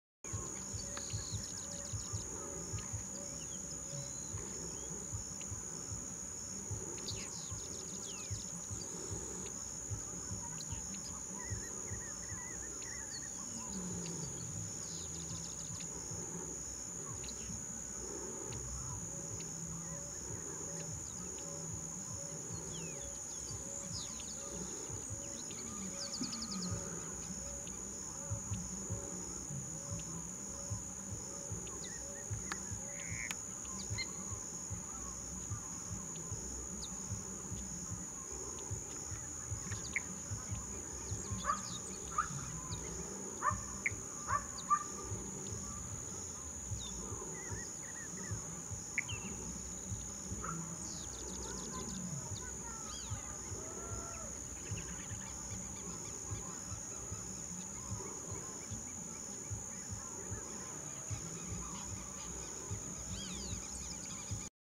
LAVADEIRA DE CARA BRANCA (Fluvicola albiventer)
Lavadeira-de-cara-branca
Nome Científico
Fluvicola albiventer
(Spix, 1825)
Nome em Inglês
Black-backed Water-Tyrant
A lavadeira-de-cara-branca é uma ave passeriforme da família Tyrannidae.
Nome Científico

Seu nome científico significa: do (latim) fluvius,fluvii = rio; e cola, colere = morador, habitante, que habita; e do (latim) albus = branco, alvo; e venter, ventris = barriga, ventre. ⇒ Pássaro ribeirinho de barriga branca.
Características

Mede 14 centímetros. Apresenta coloração preta nas partes superiores, com a parte posterior da coroa, nuca, manto, asas e cauda pretas. Testa, face, peito, ventre e crisso são brancos. As asas possuem duas barras alares brancas estreitas. Íris, bico e pernas escuras.